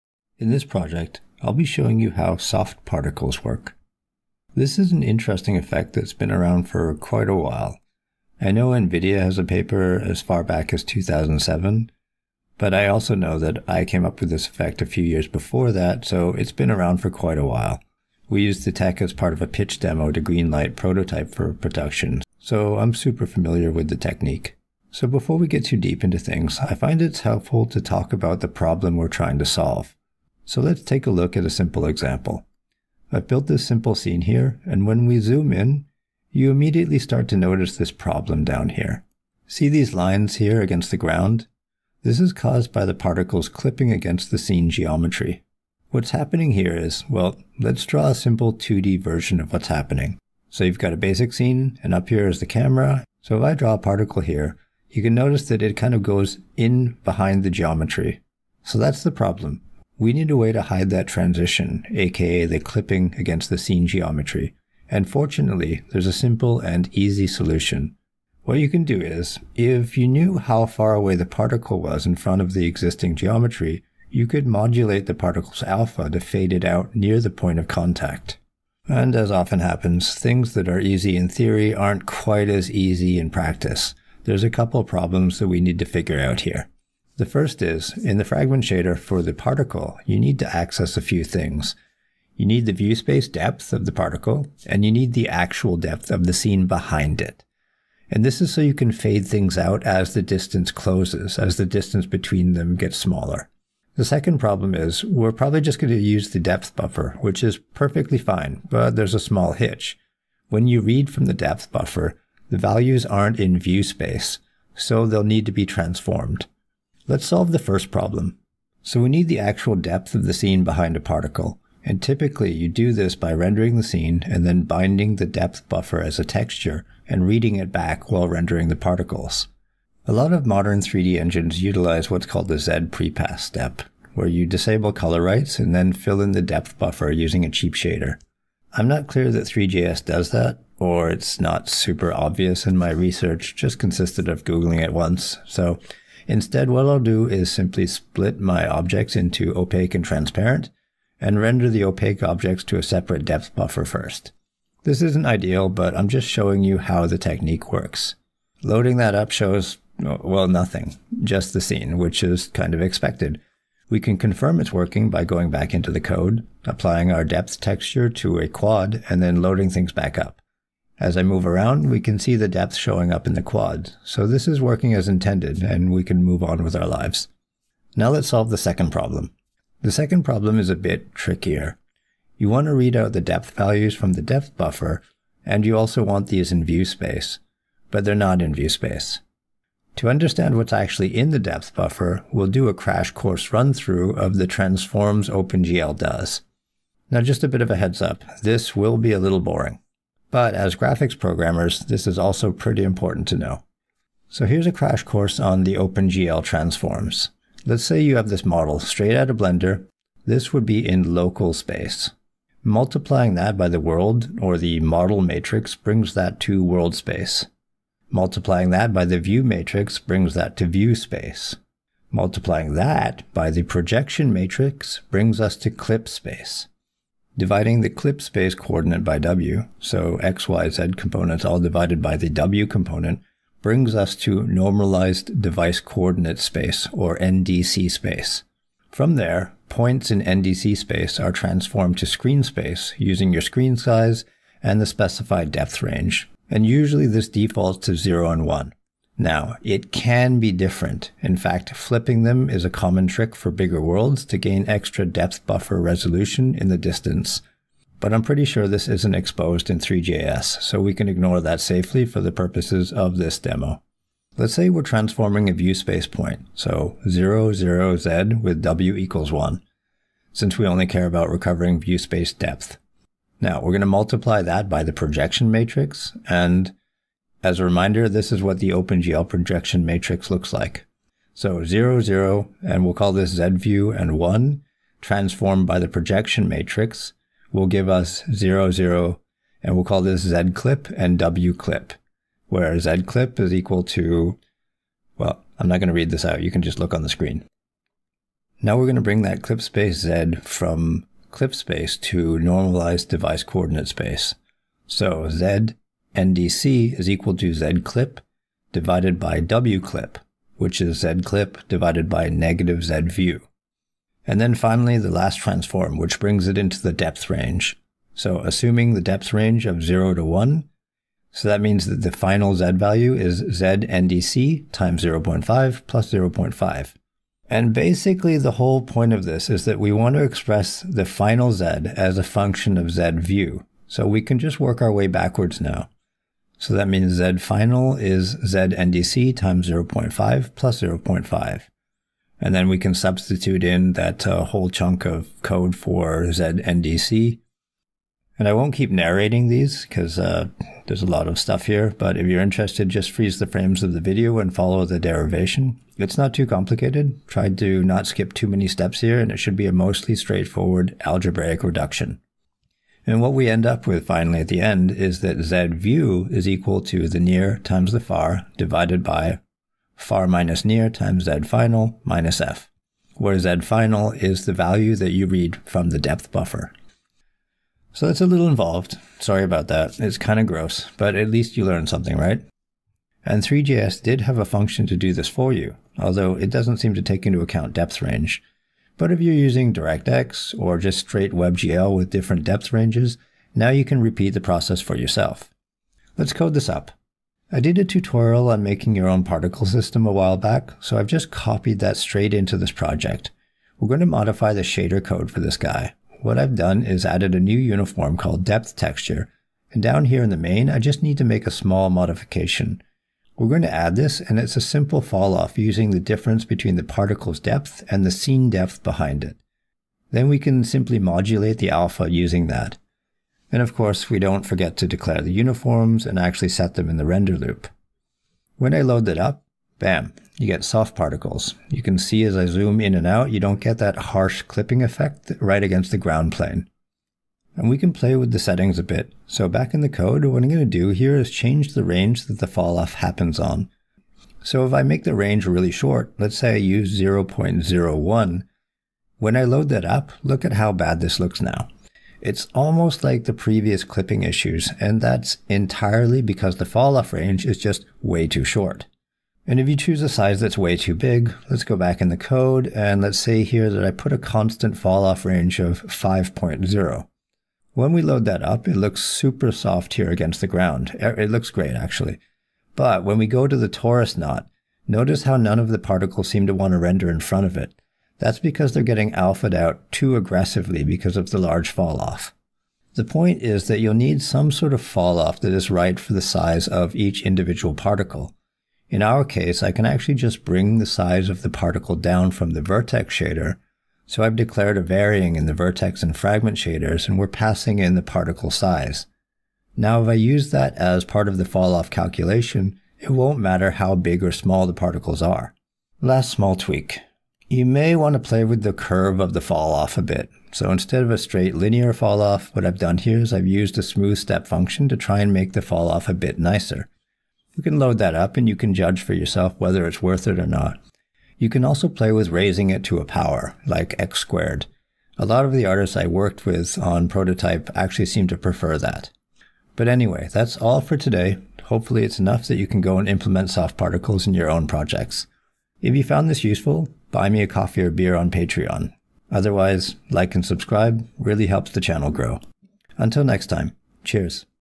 Fluffy, Soft Particles (WebGL/Three.js)
In this project, I'll show you how soft particles work, and walk you through the opengl transformation pipeline.

We'll be looking at adding soft particle support in three.js and JavaScript. This tutorial covers how soft particles work, how to structure your three.js or webgl program to render depth values, how to read and transform values from the depth buffer, and ultimately how to write the shader code that does the soft particle effect.

This tutorial uses three.js, which is a library in JavaScript for cross-browser 3d graphics using webgl. It makes high level functionality and advanced graphics available on the web.

In the video, we cover:
* OpenGL Transformations, Local, World, View, Projection, NDC spaces
* Reading from the Depth buffer
* Fading particle using depth differences